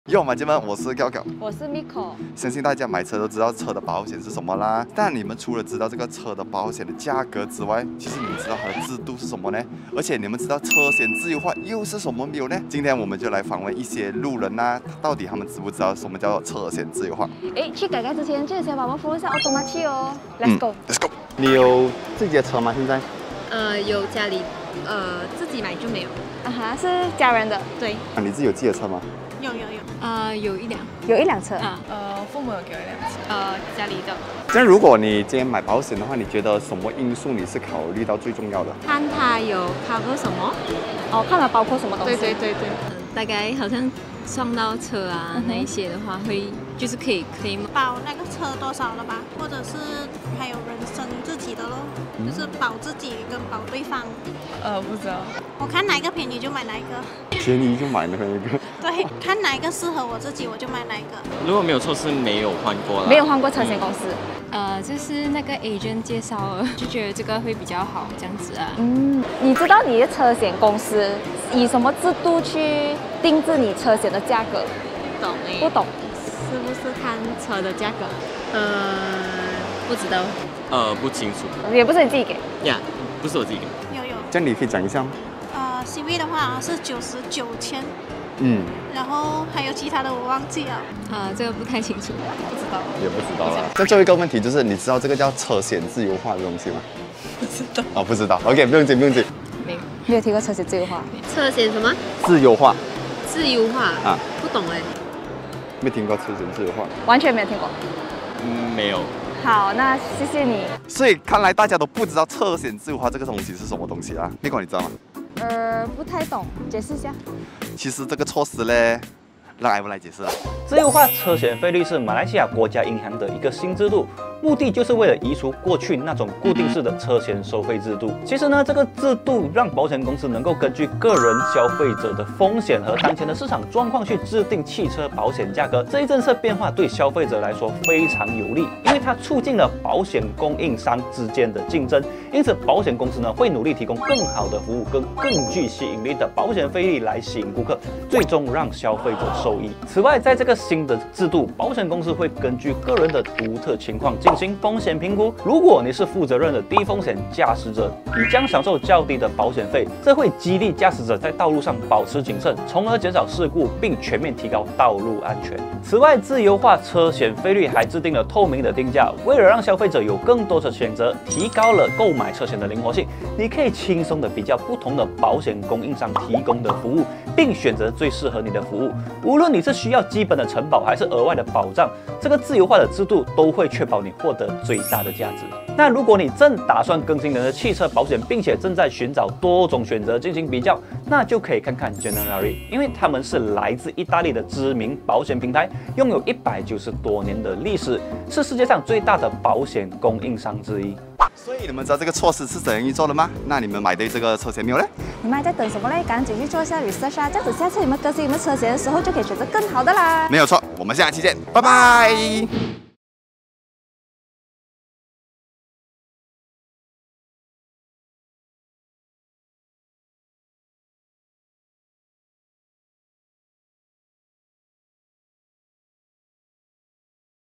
汽车保费是怎样被制定的？消费者能如何从中受惠？（汽车咖啡馆）｜automachi.com 马来西亚试车频道
相信很多人在购买车险时都会贪图方便，不会考虑太多。

但你知道每年缴付的车险保费，不只是以车型和车款定制的吗？

 其实目前我国已经实行了自由化车险政策。各家保险公司为了确保自己的竞争力，都会尽可能为消费者提供更多更好的服务，并确保所提供的车险价格具有优势。

 因此在购买车险之前，如果你能够货比三家的话，那不但能找到最适合的车险，还可能省下不少保费哦！